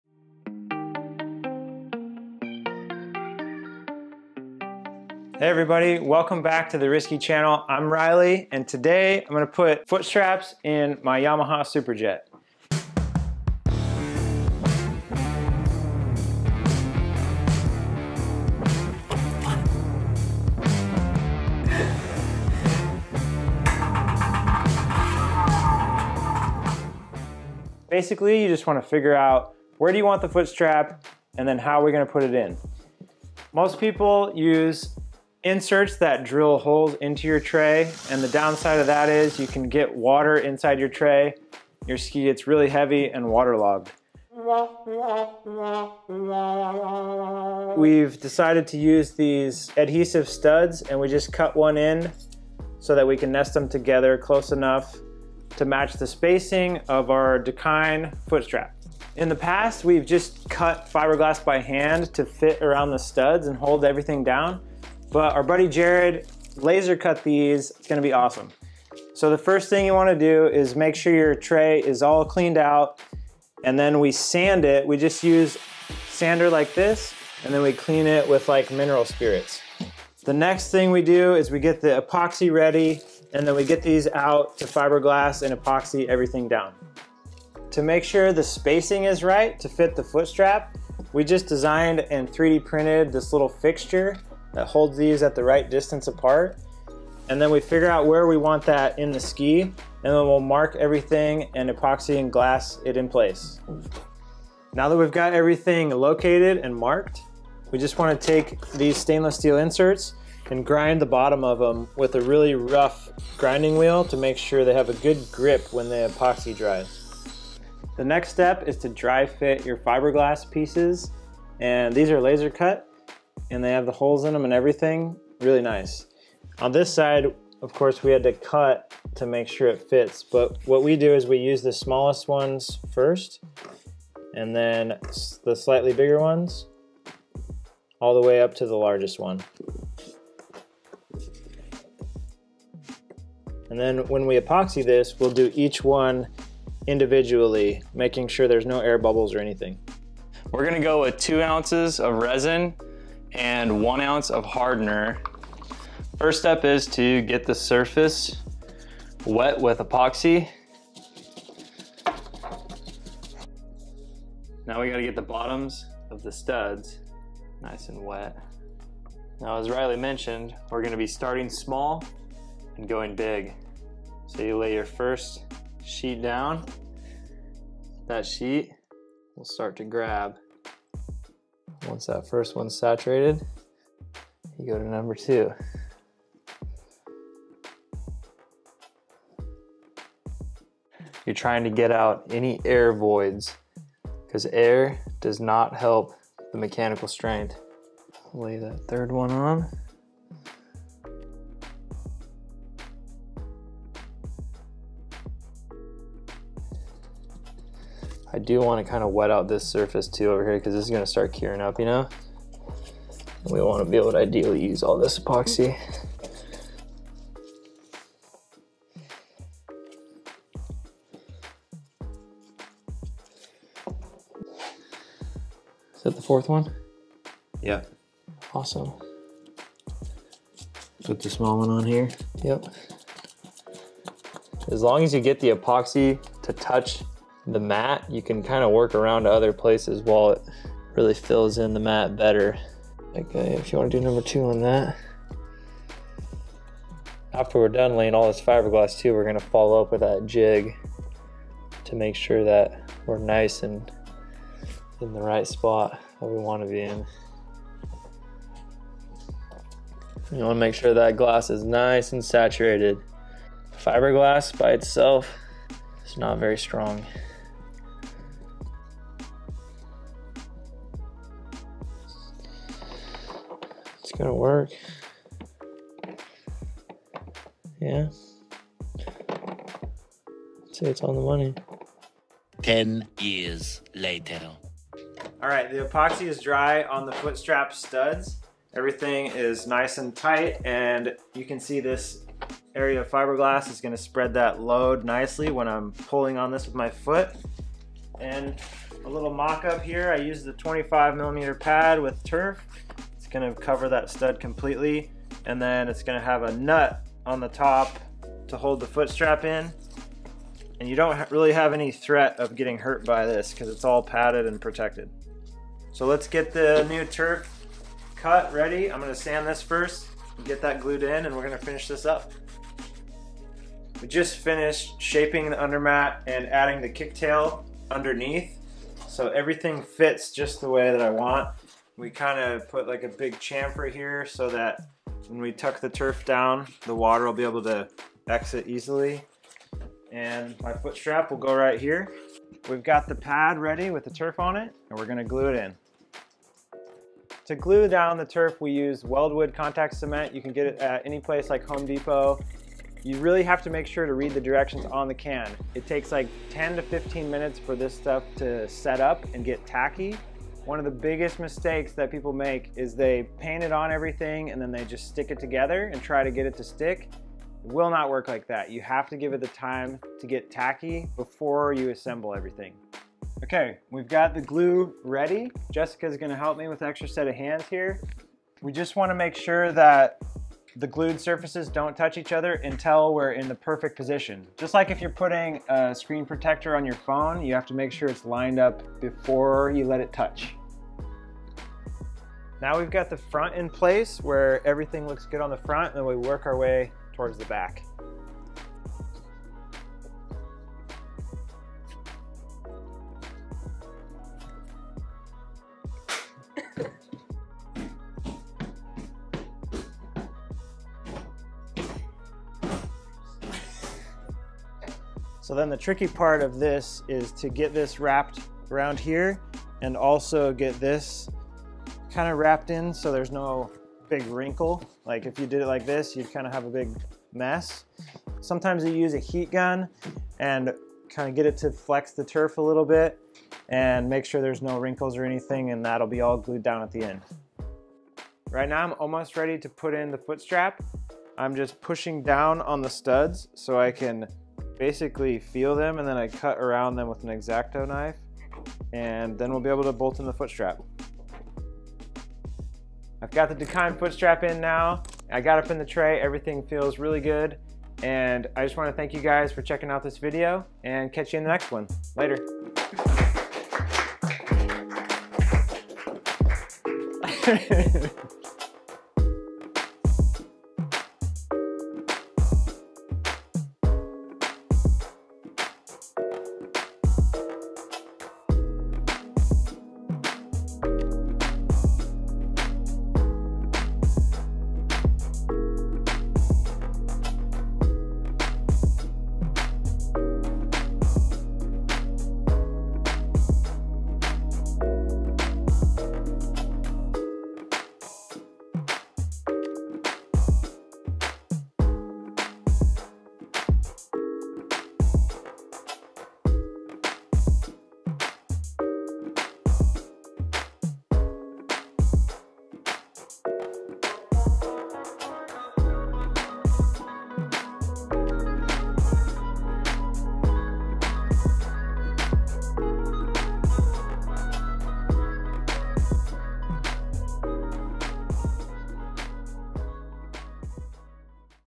Dakine Footstrap DIY Install for Yamaha Superjet Stand Up Jetski with RSKI RIDERS
Join us as we show you how we install Dakine footstraps in all of our jetskis!
The link to where we buy the adhesive studs and nyloc nuts:
Have fun!